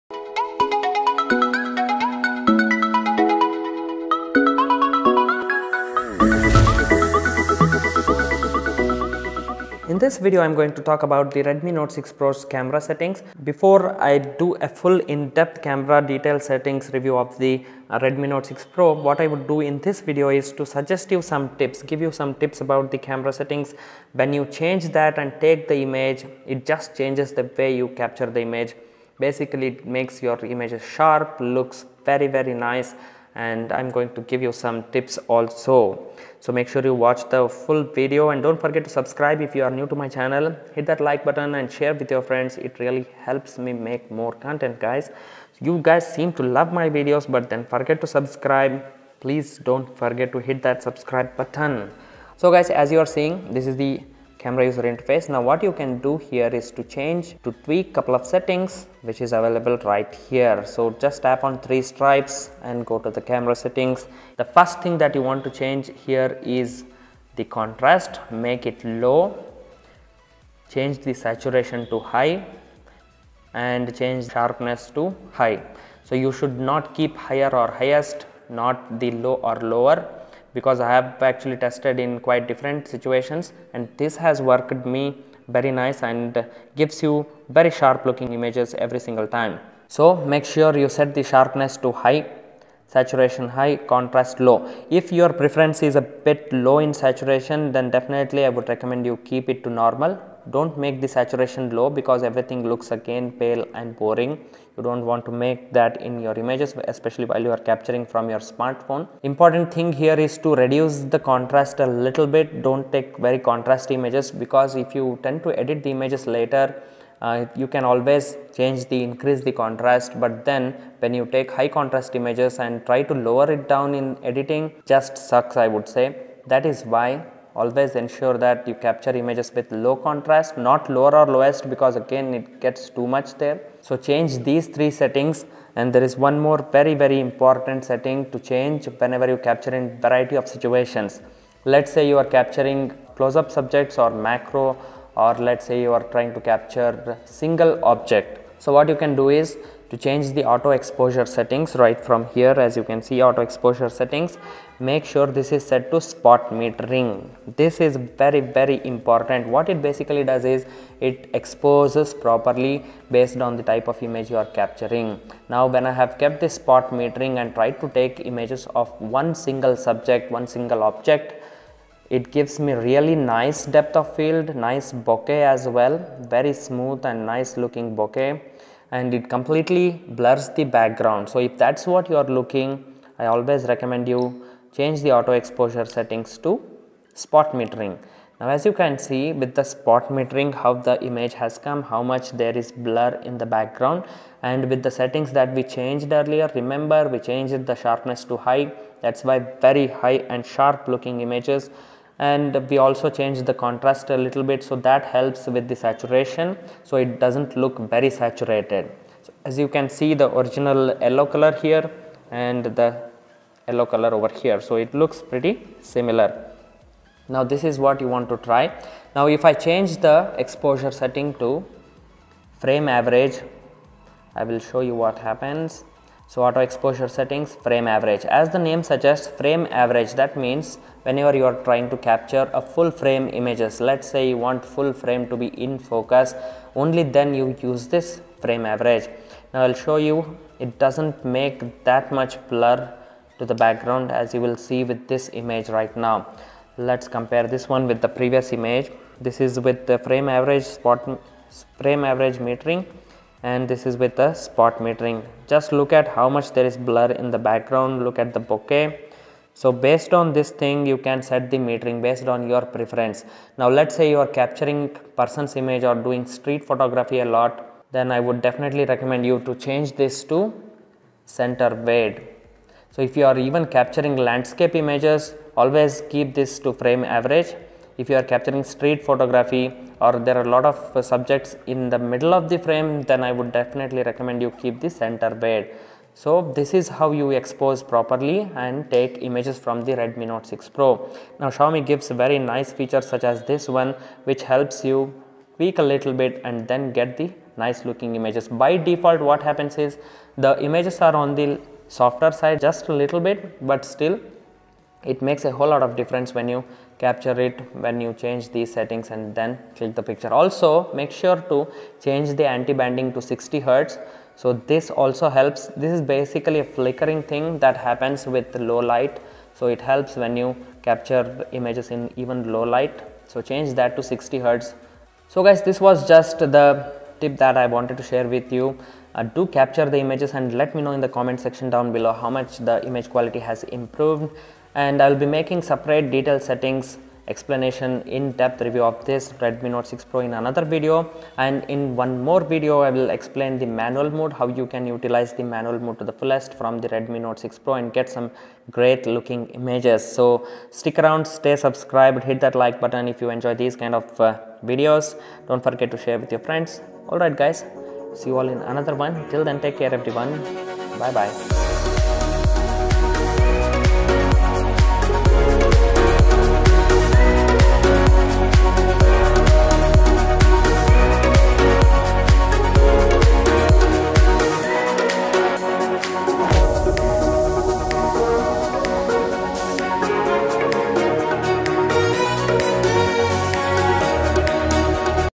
Redmi Note 6 Pro Camera Tips | Important Camera Settings
In this video I will share important Redmi Note 6 Pro camera tips which helps you take better images. As you know, redmi note 6 pro takes nice images but there are some camera settings which when tweaked a little bit, the redmi note 6 pro camera produces awesome sharp looking images.
Redmi Note 6 pro has a 12MP - f/1.9 + 5MP - f/1.9 Aperture dual camera setup. The 5 MP camera helps blur the background along with primary 12MP camera setup.
Xiaomi gives nice features for camera, such as adjusting the exposure settings, metering and more which is very well implemented in the Redmi note 6 pro and I have explained you how to use it properly and take fantastic pictures.